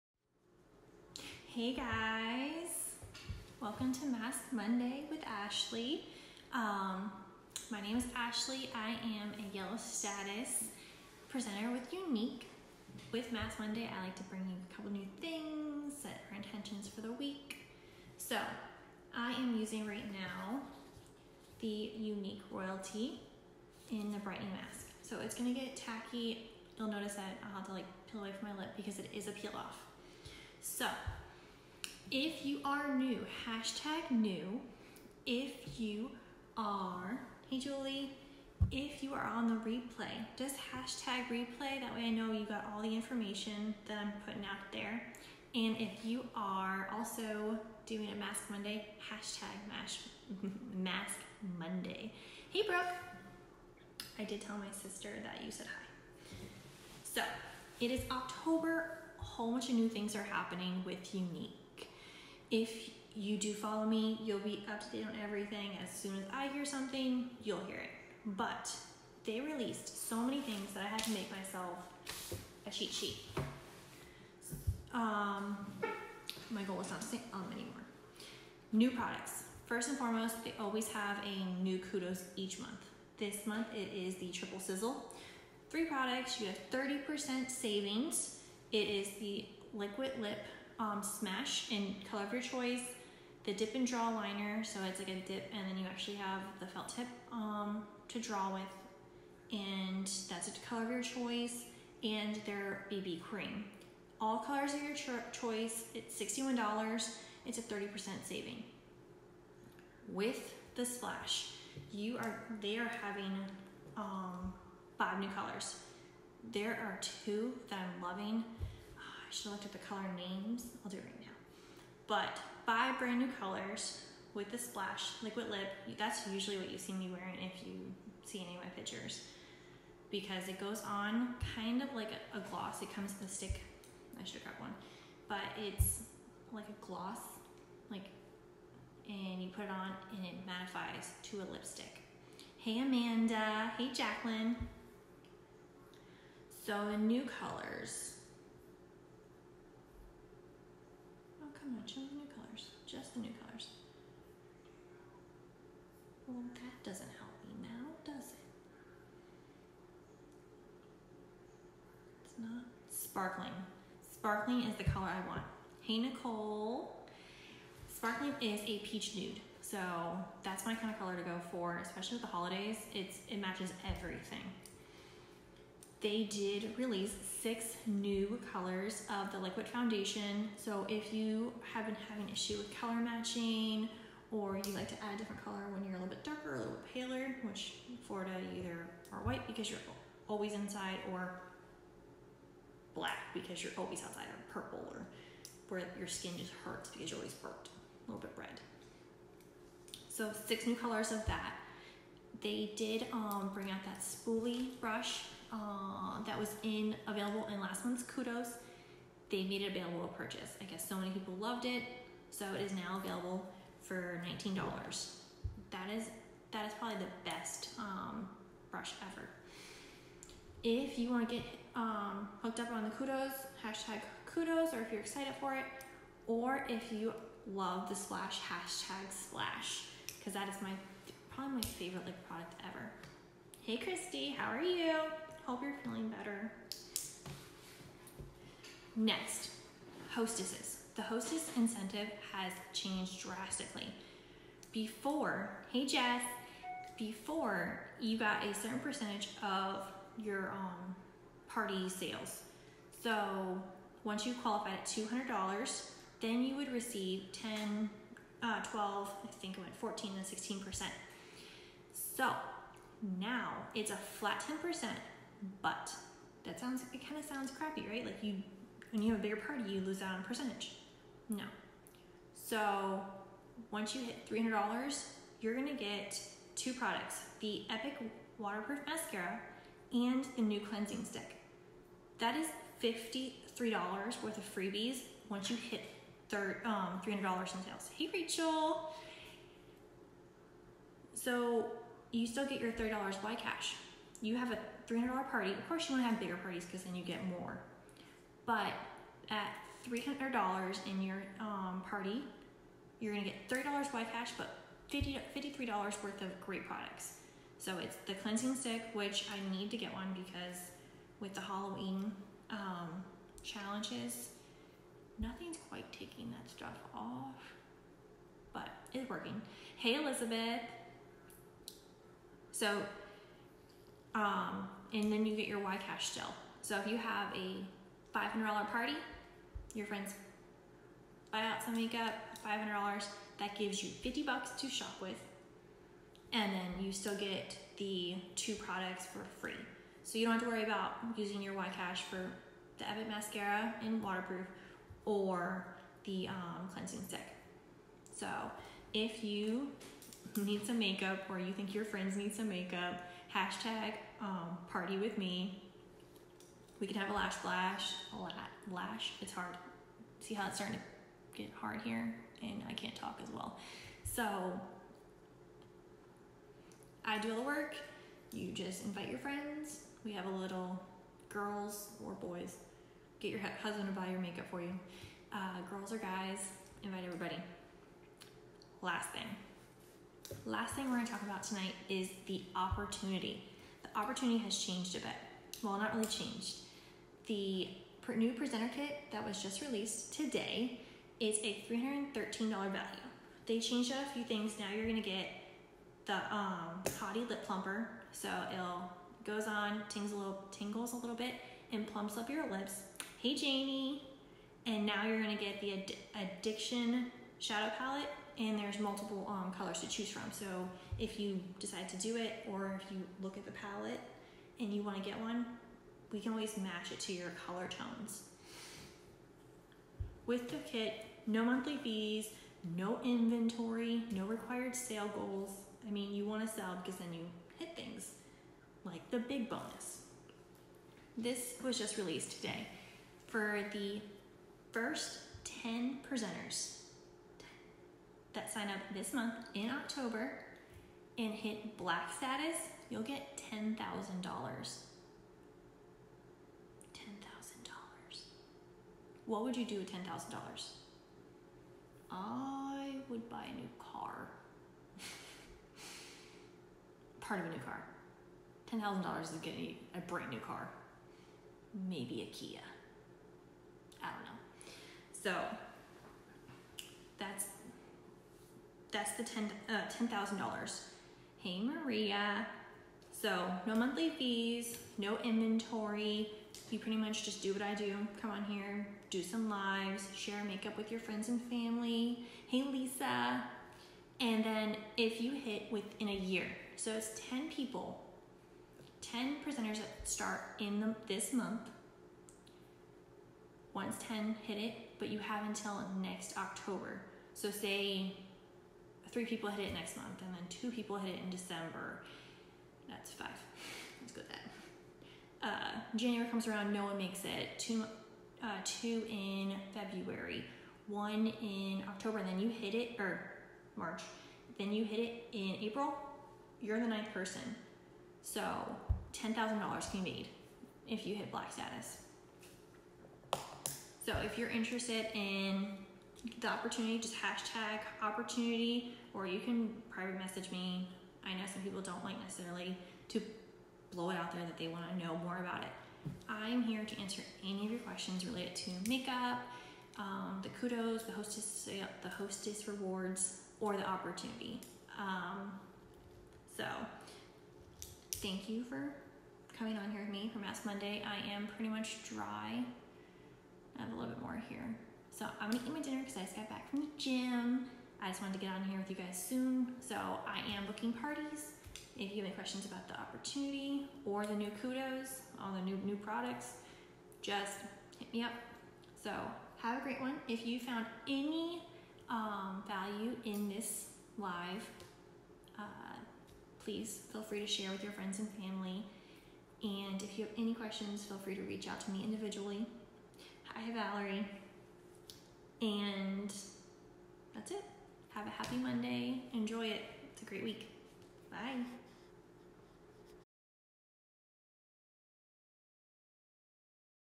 Younique October 2018 Launch
Younique Launched Lots of New Products and Incentives this Month

**New Products include 5 New Shades of Splash Liquid Lipstick and 6 New Shades of Touch Mineral Liquid Foundation

**New Hostess Incentive includes a Free Waterproof Epic Mascara and Cleansing Stick (Book your Party today

** New Opportunity Incentives include New Products in the Kit Plus a chance to earn $10,000 ( Learn about the opportunity

Comment with an suggestions or questions, I'm here to try and answer :)